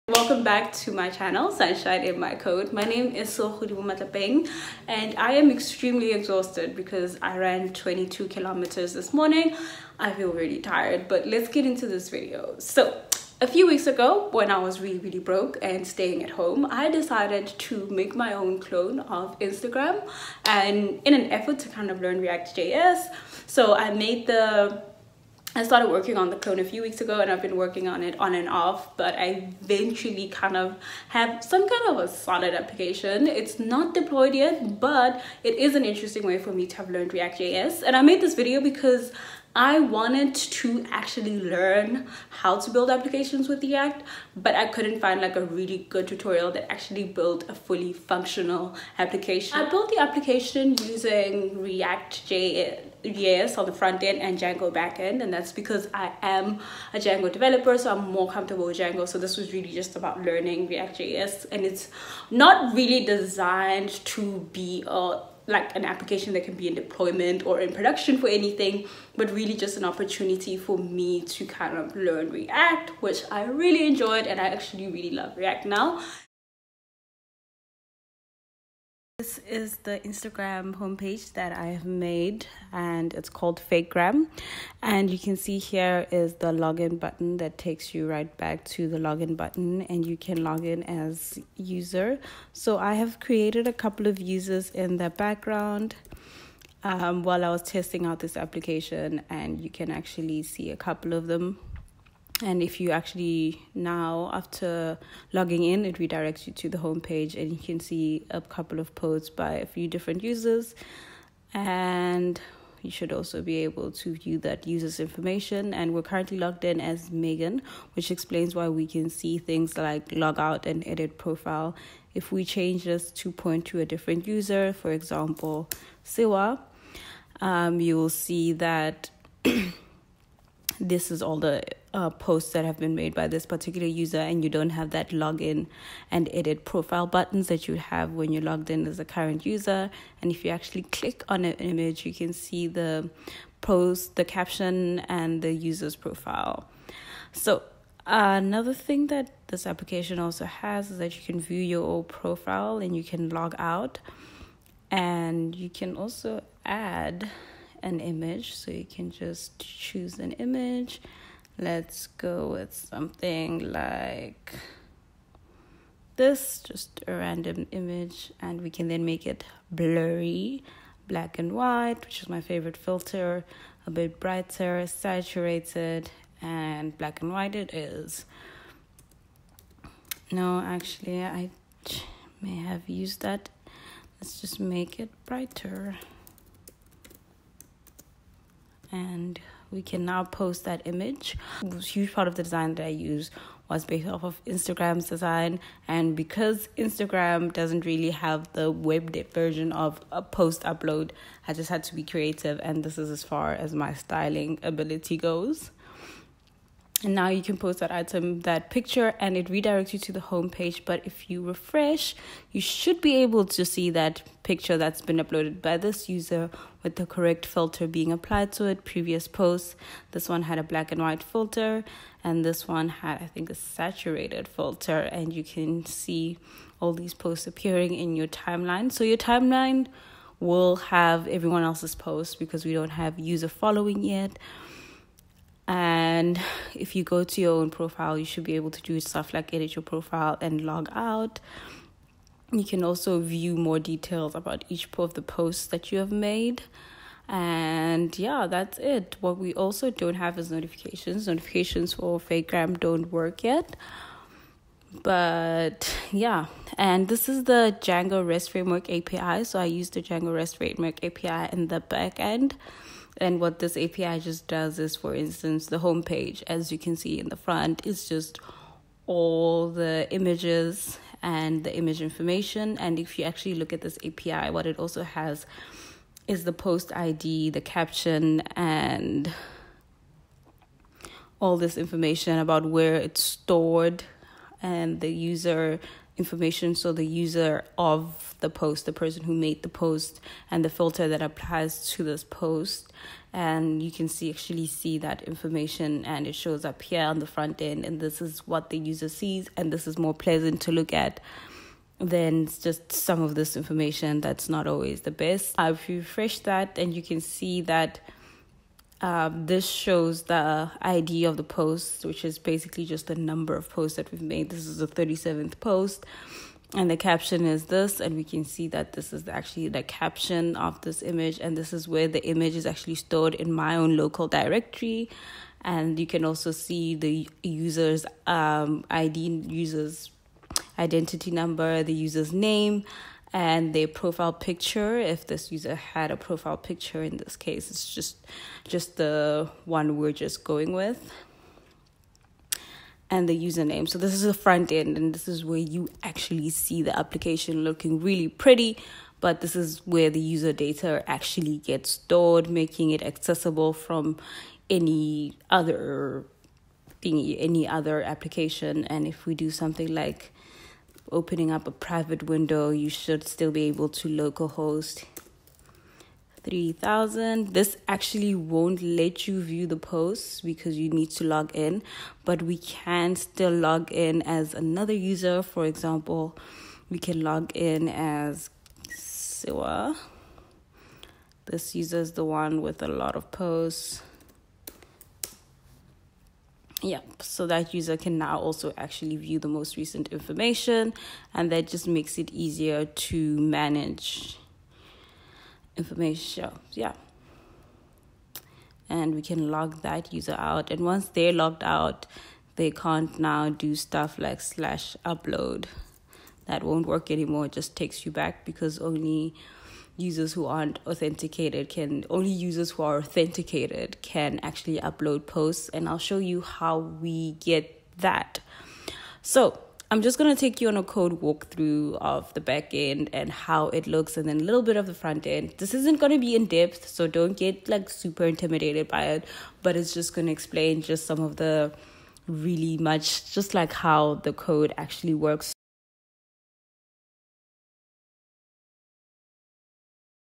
Instagram Clone built using ReactJS and Django Rest Fullstack | Sunshine In My Code
I made a clone of the an the Instagram Application using ReactJS frontend and Django Rest Backend. It was my first time using reactJS to create an application. In thi video we will just do the app walkthrough and the next one will be the code walkthrough